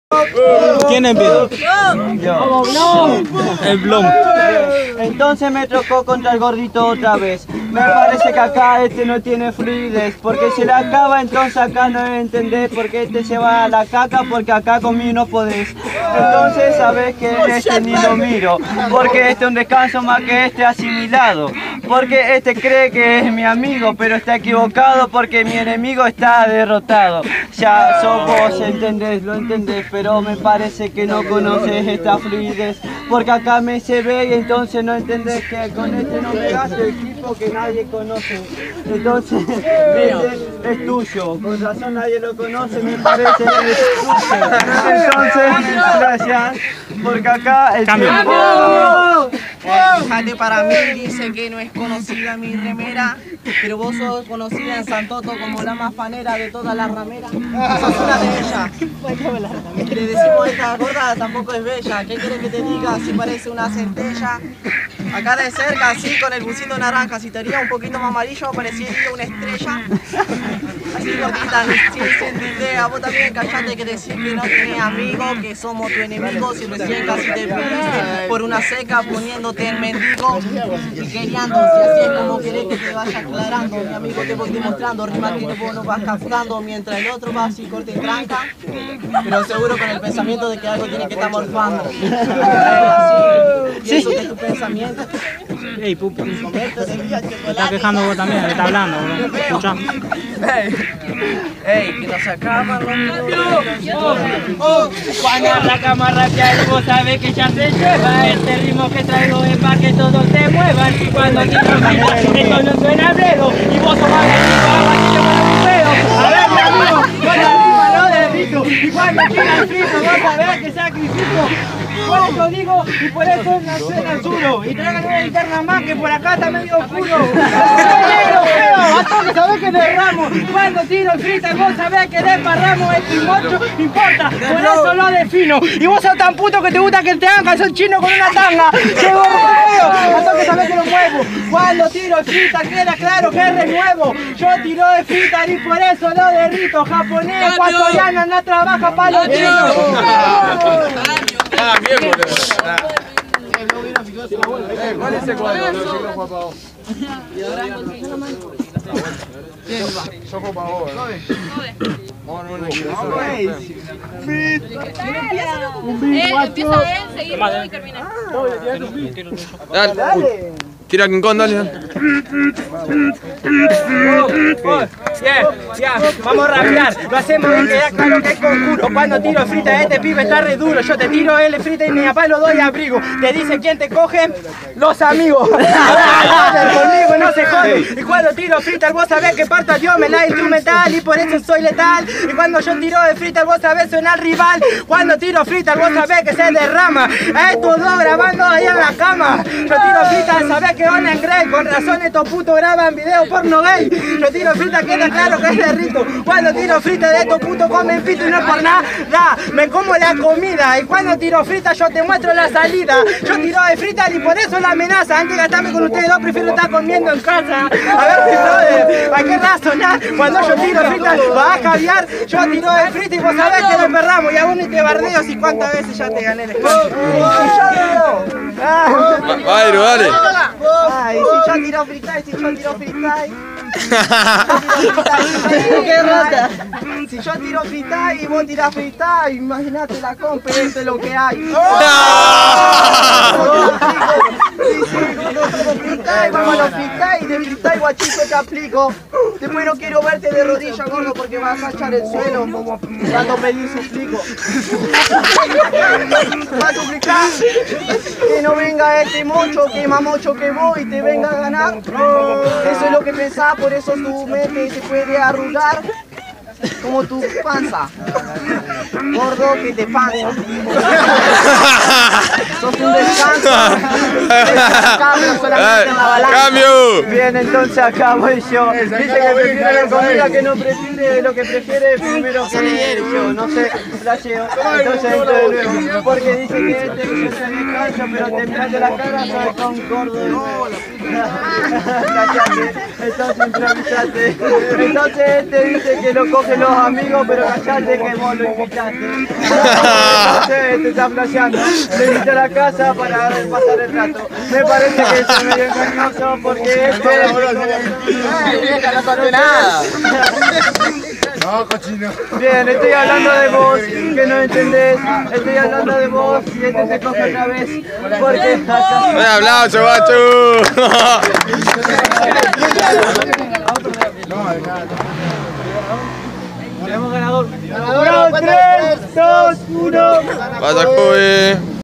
Cobe vs Blow vs Airuf 8vo Fridaay Feestyle 2da Temporada
Fridaay Freestyle 2da Temporada

Modalidad: 1 vs 1

Lugar:Santa Fe,Santo Tomé costanera(anfiteatro)

Ganador: Cobe